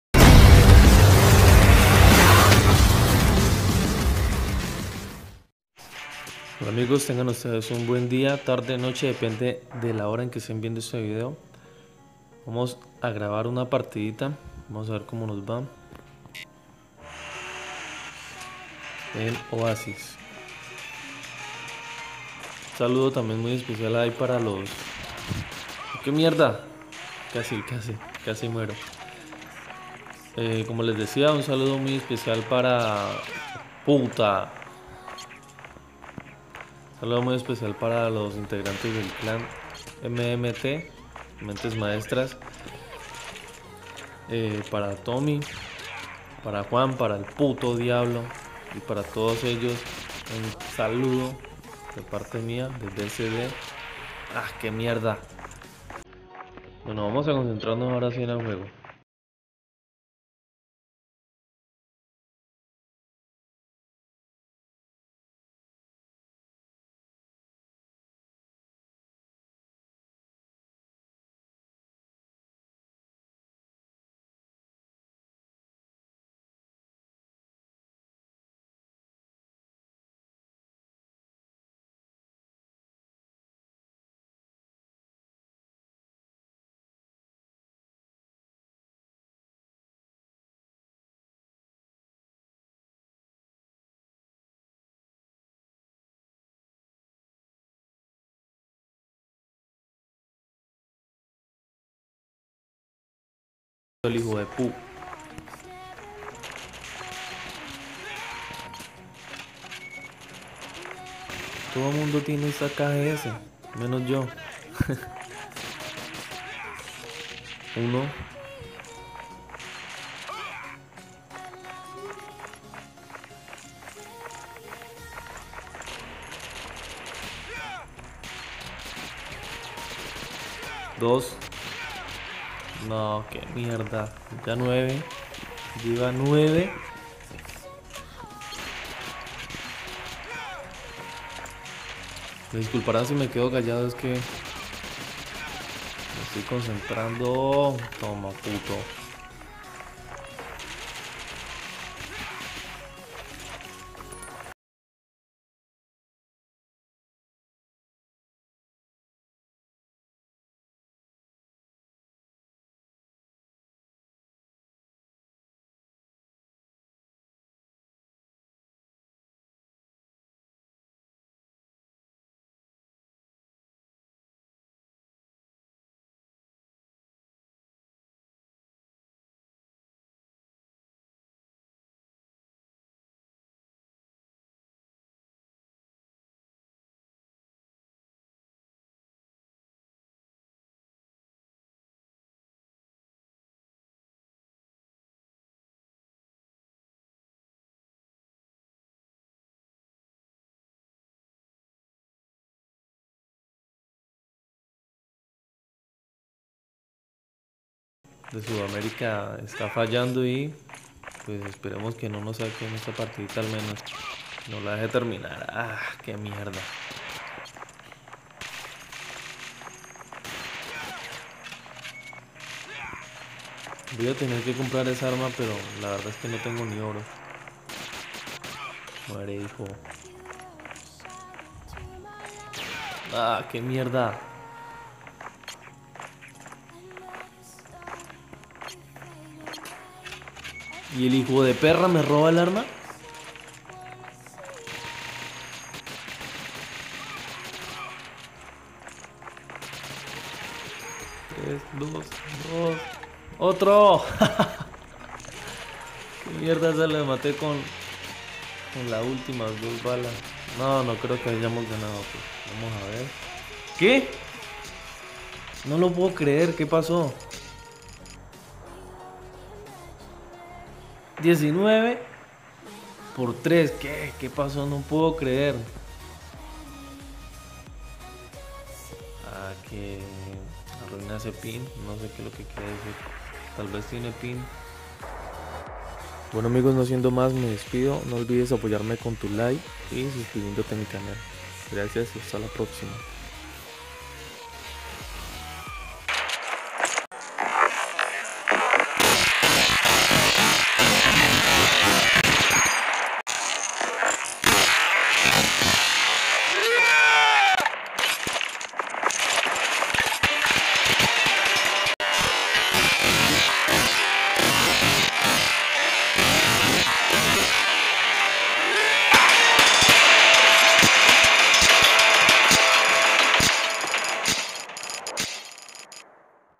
Una dura partida con final inesperado!
Cuando me veía acabado, tuve la necesidad de convertirme en super saiyajin 😃 .  Espero les guste el video.

Gracias